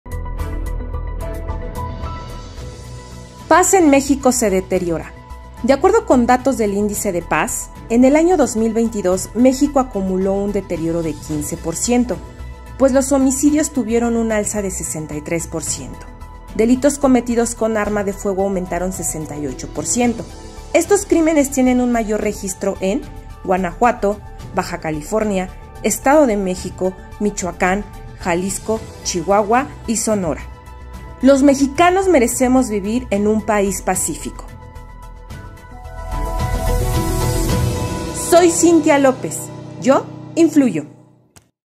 Paz en México se deteriora
El Instituto de Economía y Paz publicó la 8.a edición del informe anual del Índice de Paz en México, en el que se muestra que el país es 15 por ciento menos pacífico que ocho años atrás.